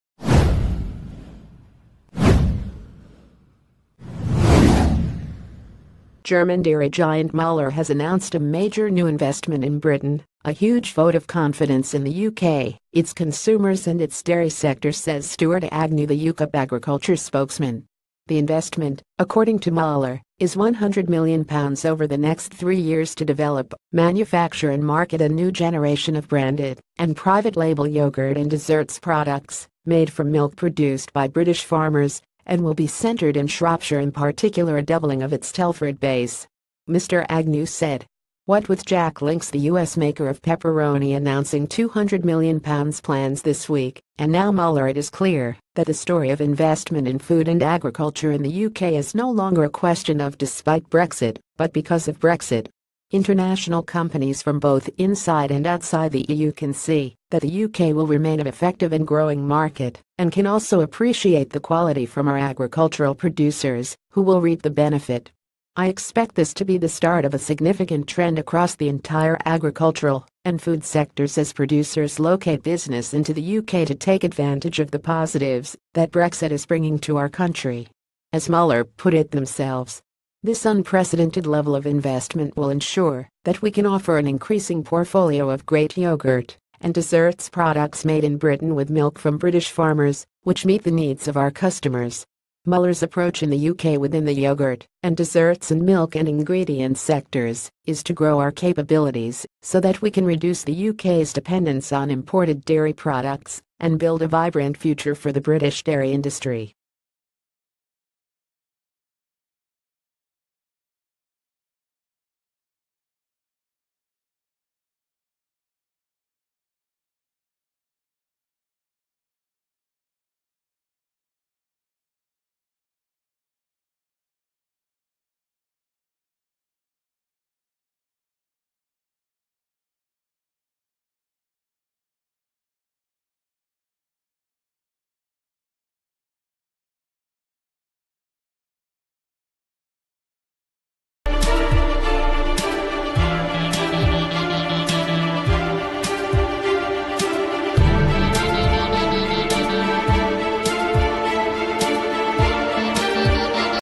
Breaking News | Wünderfull stuff, müller announce £100 million investment in britain
Wünderfull stuff, müller announce £100 million investment in britain

German dairy giant Müller has announced a major new investment in Britain, a huge vote of confidence...